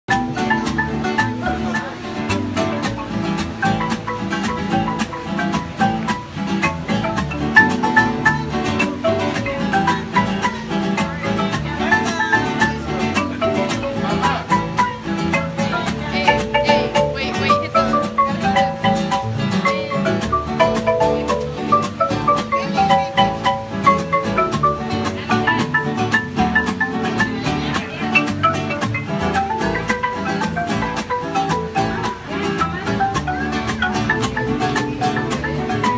Band at Epcot
Band at the Mexico pavilion in Epcot